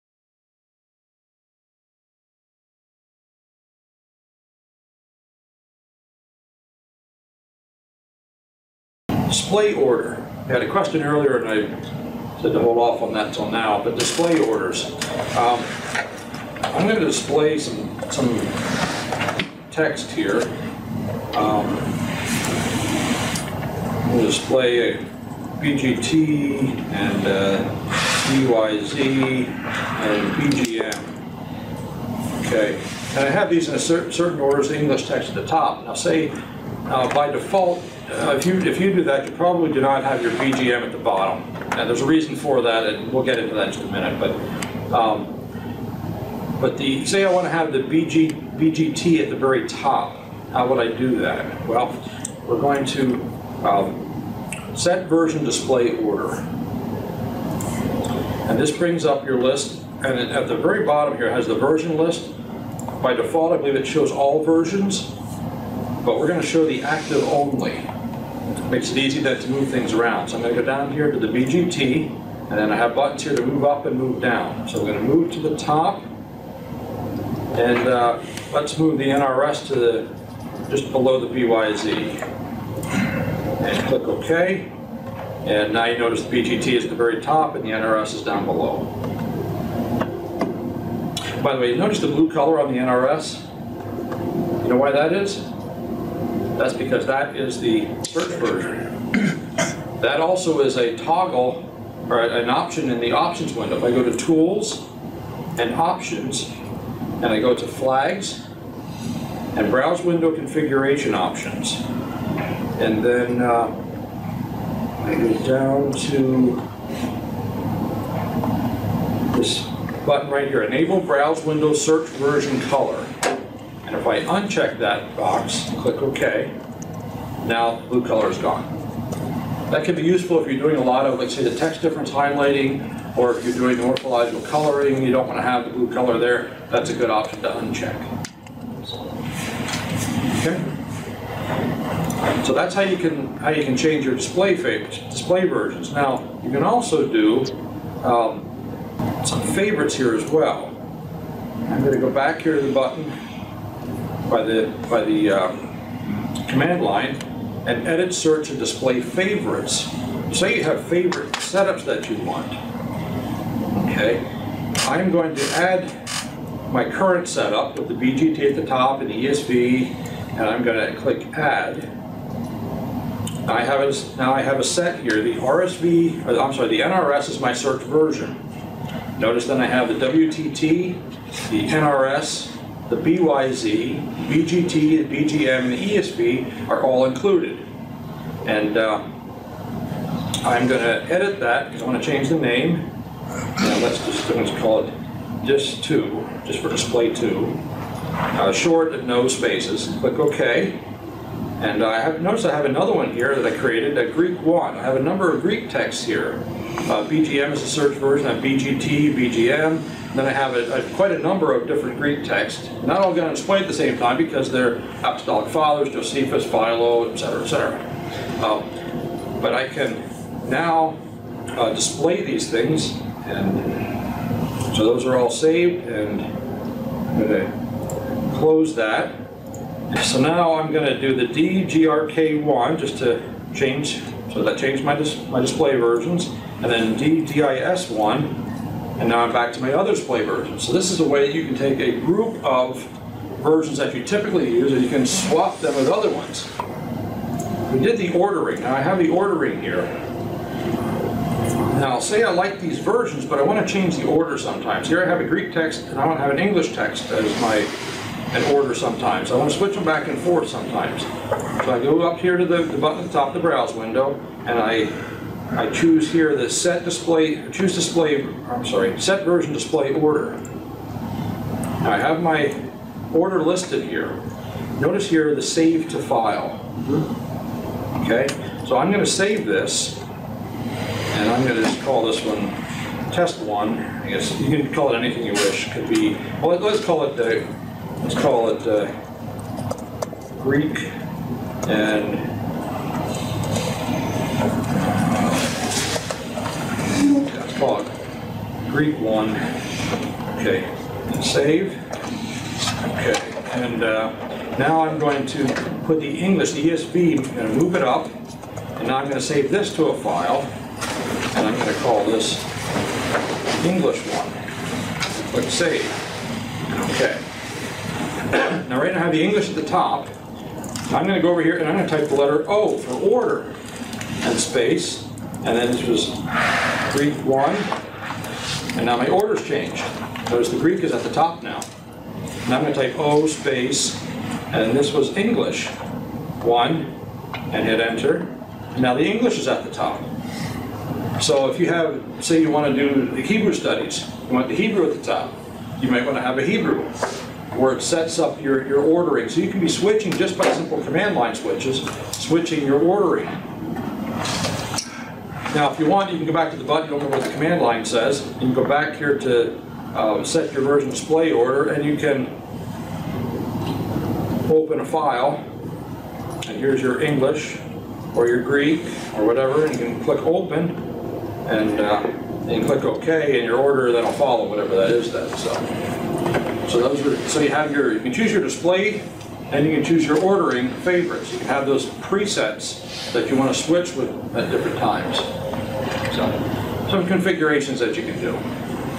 BibleWorks Workshop Part 2_6 -- Browse Window Adjustments
1. How to change Version Display Order.
2. How to turn off blue text of Search Version in Browse Window.
3. How to configure a Display Favorite of favorite versions to view in the Browse Window and open it from the Command Line with a shortcut.
4. How to save Version Display Order files and open them from the Command Line.